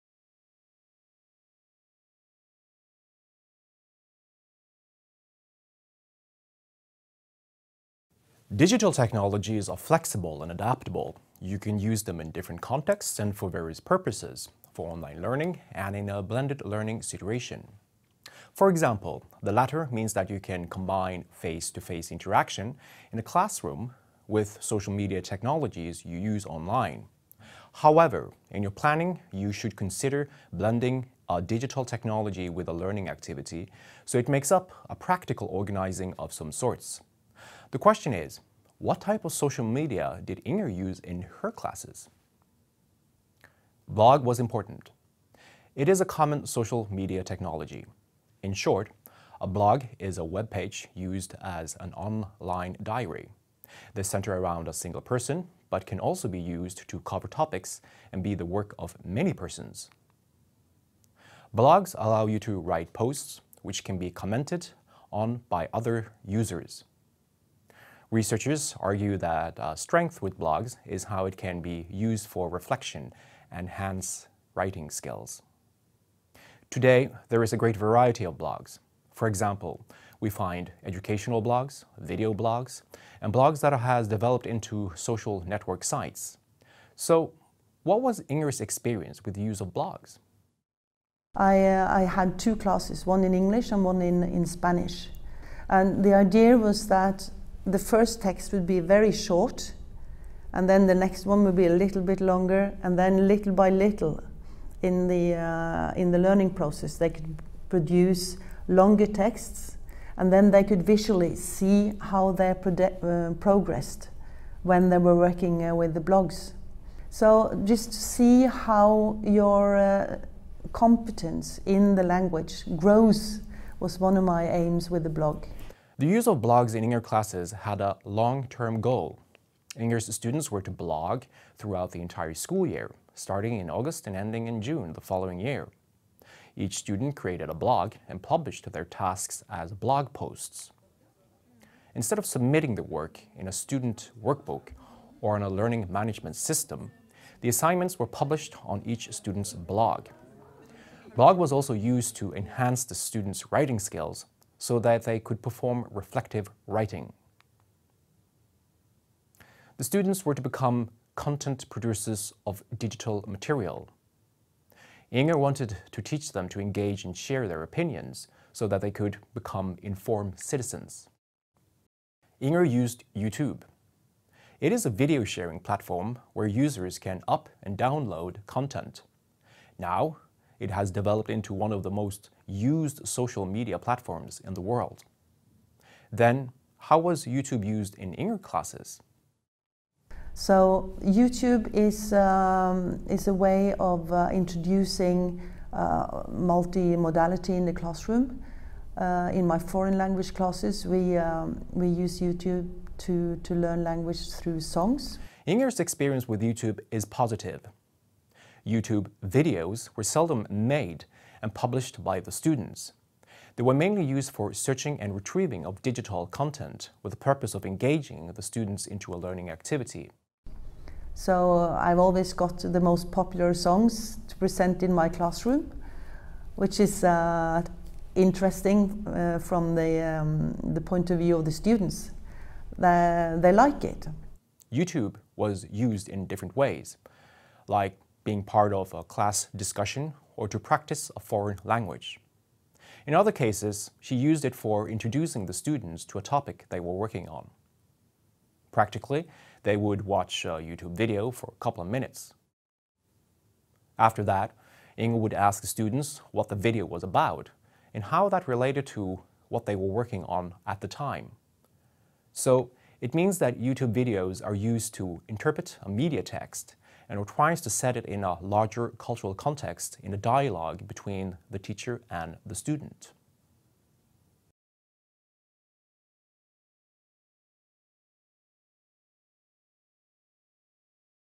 Learning design: Blog and YouTube, Digital Transformation in the Classroom
A video from the online course Digital Transformation in the Classroom that runs on the MOOC platform FutureLearn. The video explains which strategies a teacher must do when using various digital technologies. Digital technologies are required adapted to support the aim and objective behind a learning activity. The video is an example that uses the pedagogical video style talking head.